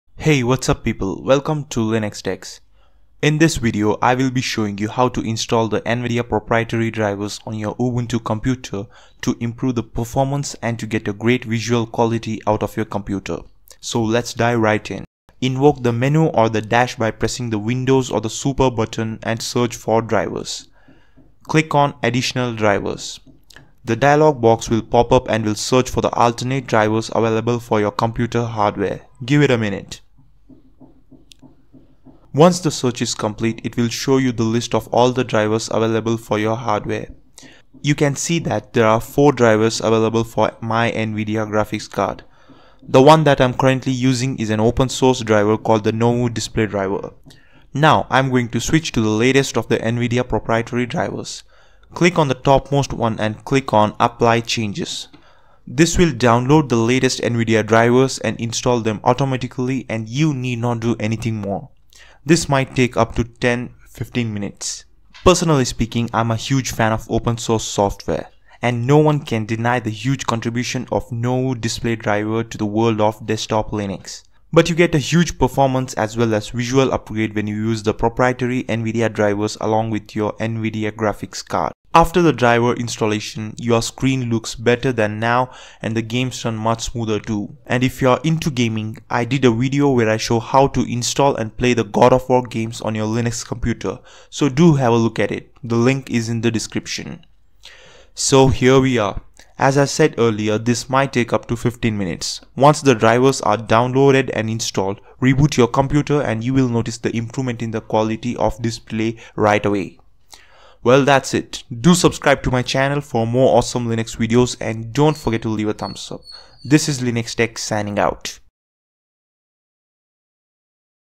How to install Nvidia proprietary graphics drivers on Ubuntu and Linux Mint
How to install and play the God of War Games on Linux.

In this video, I will be showing you how to install Nvidia proprietary drivers on your Ubuntu computer to improve the performance and to get a great visual quality out of your computer.